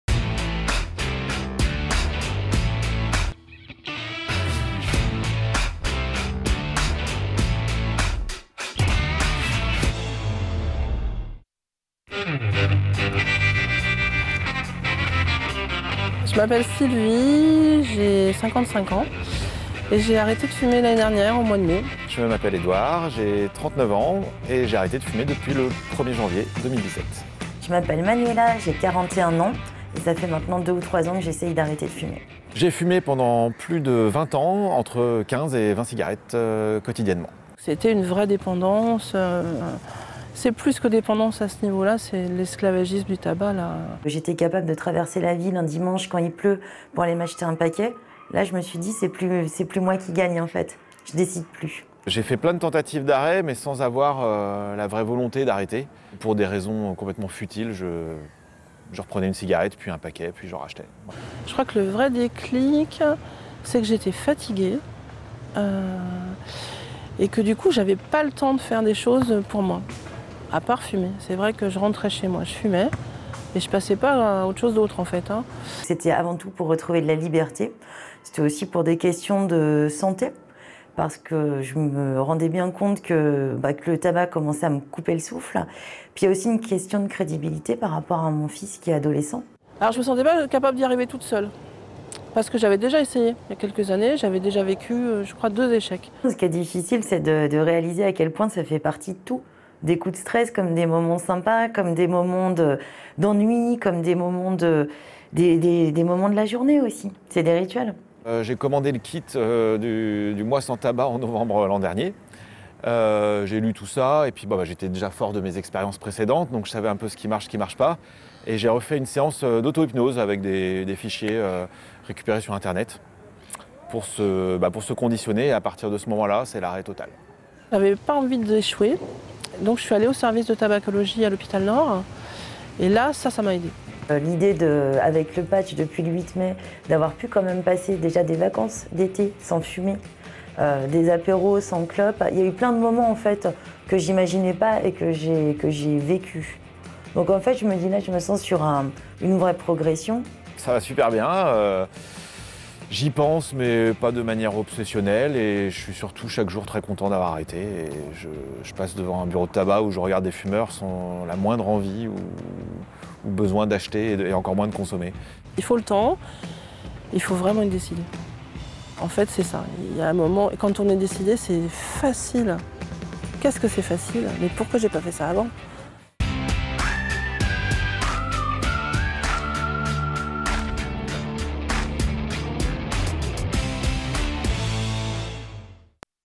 Mois sans tabac : paroles de fumeurs
Arrêter de fumer ? Ceux qui en parlent le mieux ce sont les hommes et les femmes qui ont essayé. Certains ont réussi du premier coup, d’autres non, d’autres encore sont en cours de sevrage. Chacun a une expérience singulière à transmettre, car arrêter le tabac, c’est une sacrée aventure personnelle !